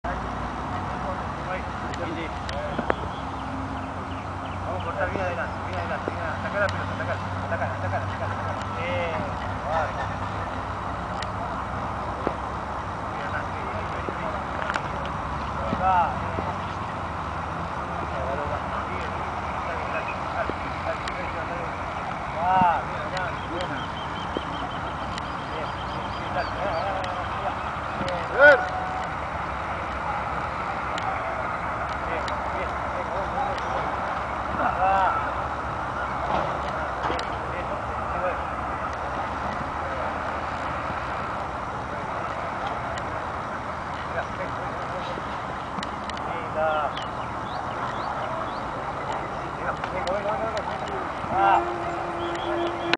Entrenamiento de arqueros - Huracán TV -
Entrenamiento de arqueros plantel profesional Club Atlético Huracán.